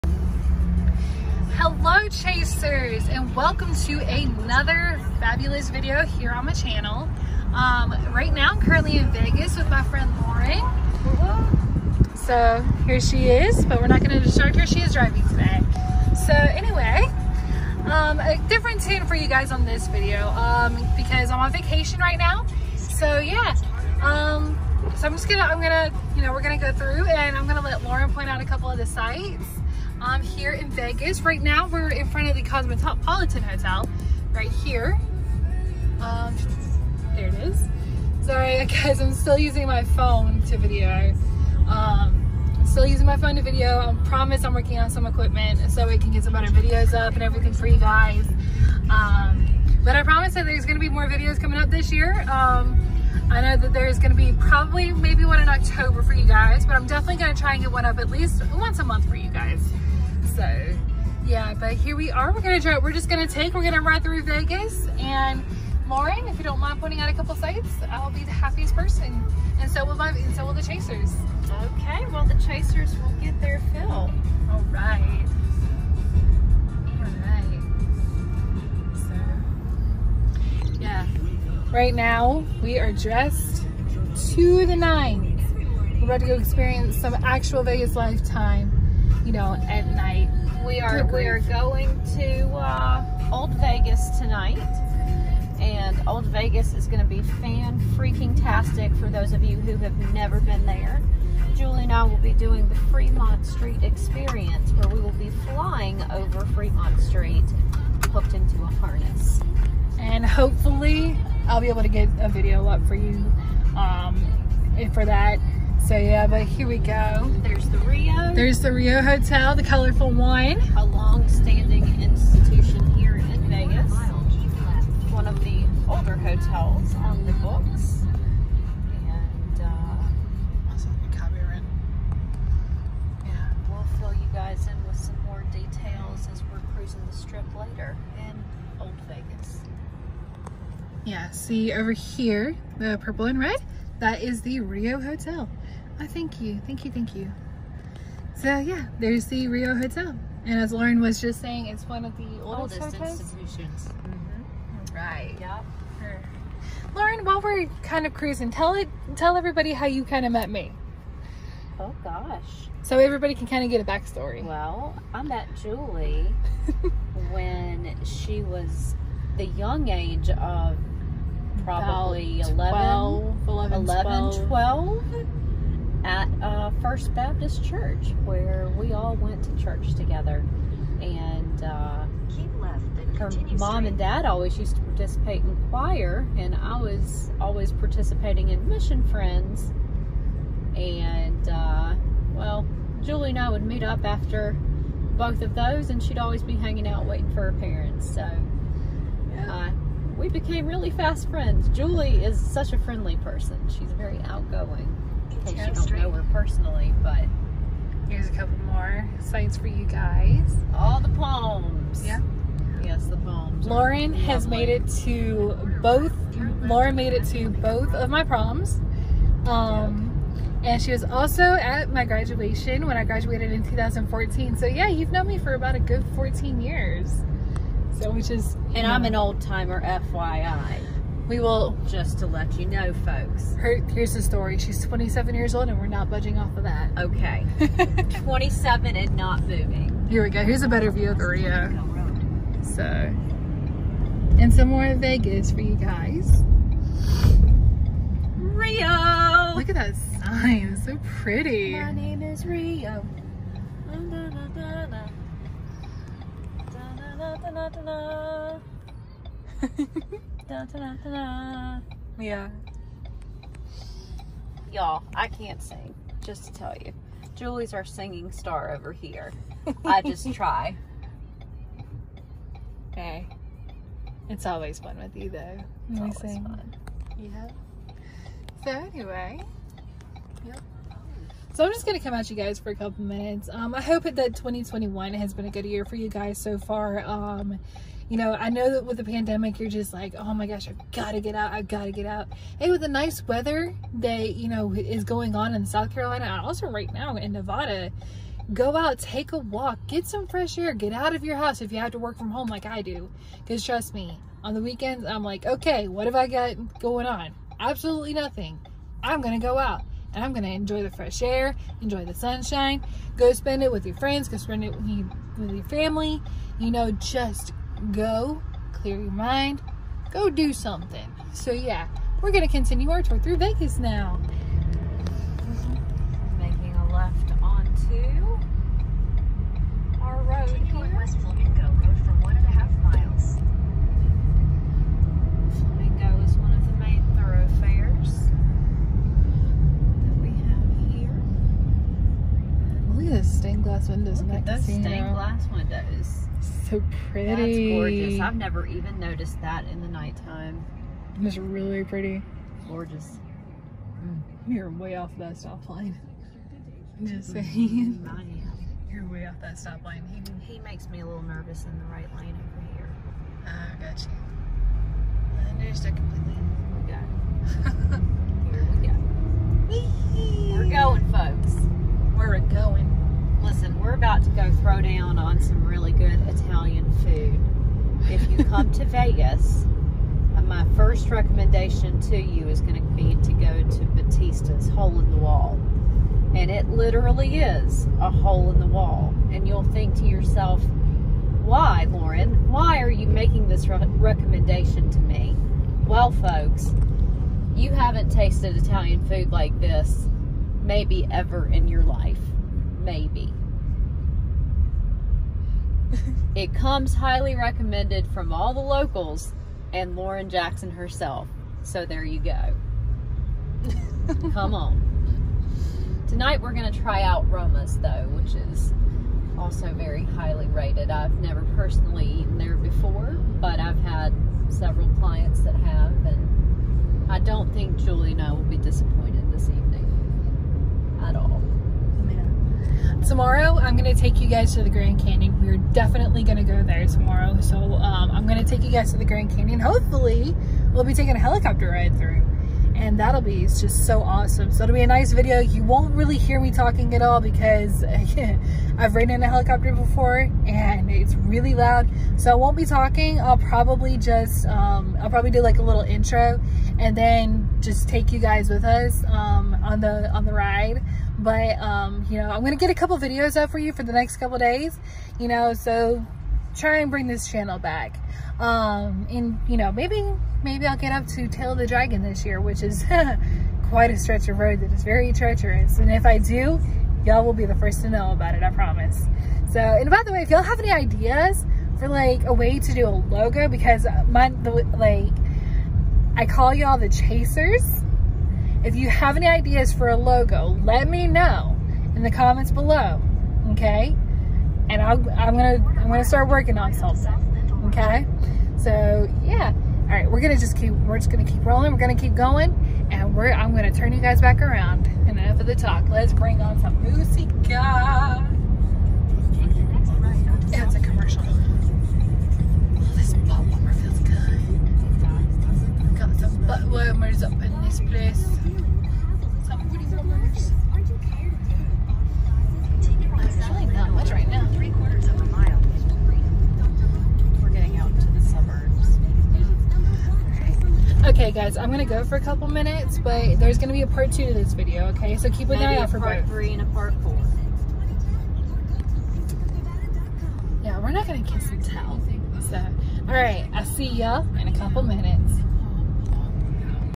Journey through Las Vegas with me pt. 1 ft. Lauren Jackson
I figured it’s time for another video so I’m switching it up. I’m on vacation right now out in Las Vegas. Love and music to you.